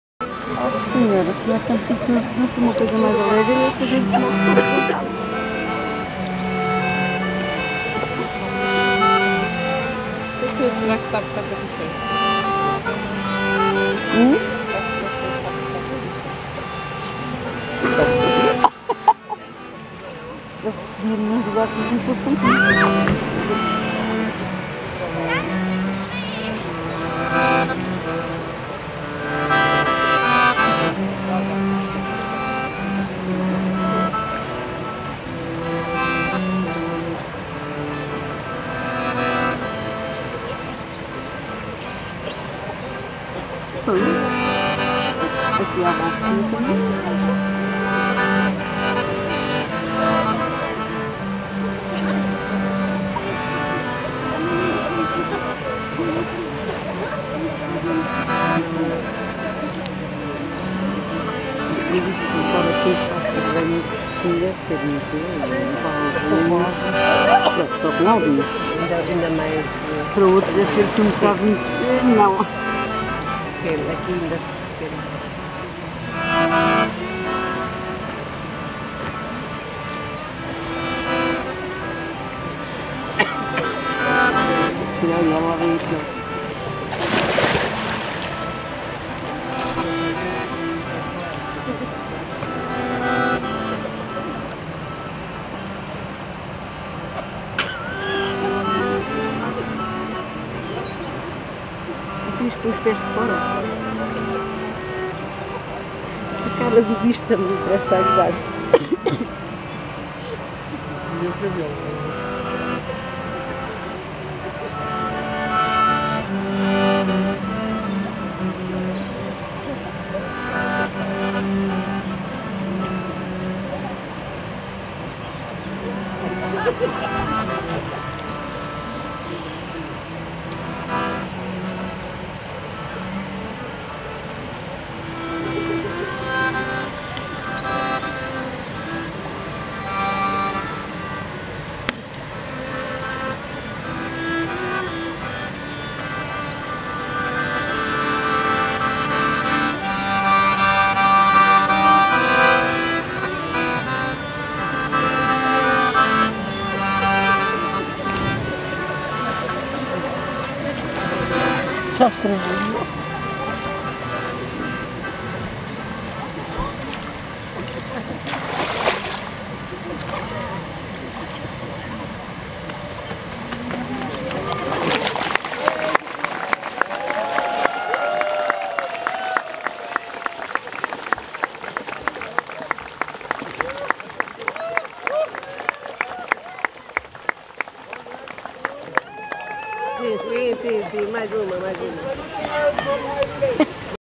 Raíz d’Aldeia 2018 - no rio 1
Festival Raíz d’Aldeia 2018, em Janeiro de Cima (Fundão), de 21 a 24 de Junho.
BALL-Trad a 24 de Junho de 2018.